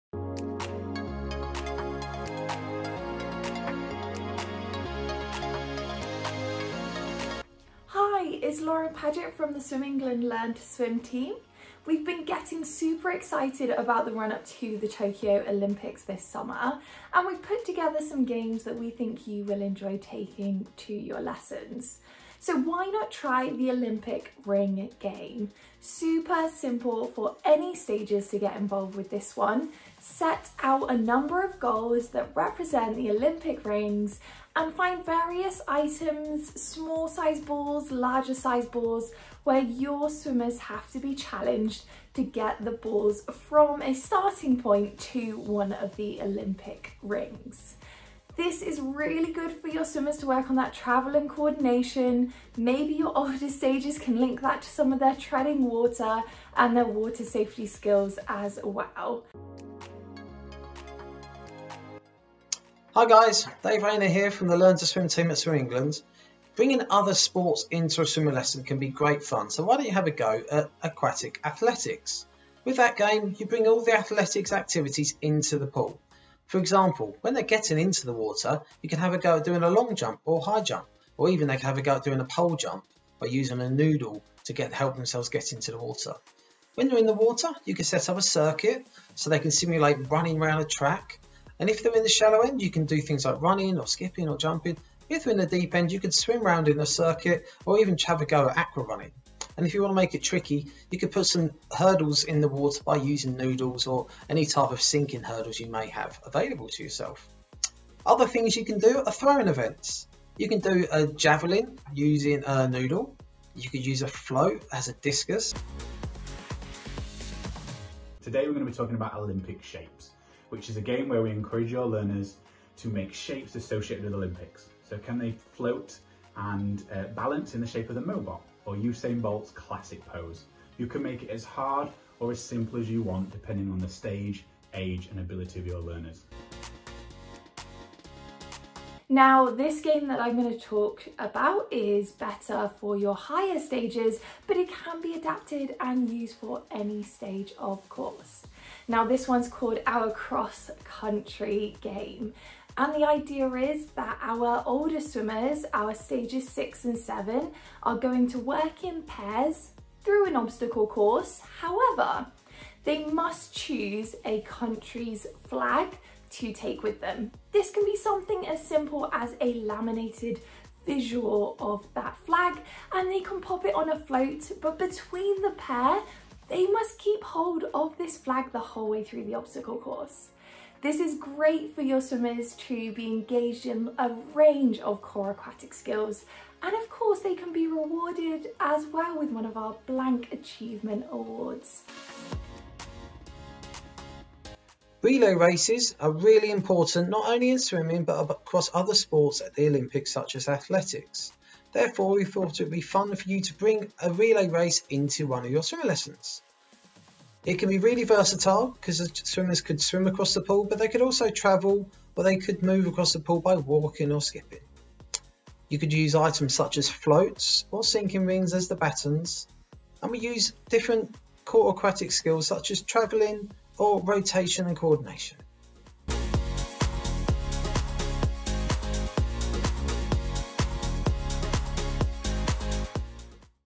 Tokyo-inspired Activities for swimming lessons from the Learn to Swim team
Our Swim England Learn to Swim team share some fun Olympic-themed activities you can take to your swimming lessons this summer.

This provides more detail on each of the activities from the video and it aligns with our Learn to Swim Framework (Stages 1-7) and the FUNdamentals stage of our Athlete Development Support Pathway (ADSP).